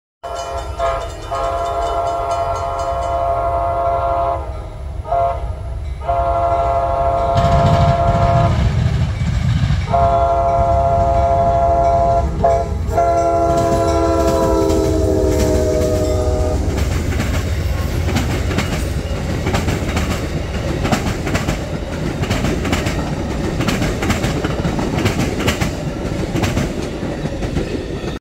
Amtrak P092 With The Big Game Unit Leaving Tampa, At Plant City 7/29/23
One of the "meh" units from Amtrak. I was there so I figured why not take the video.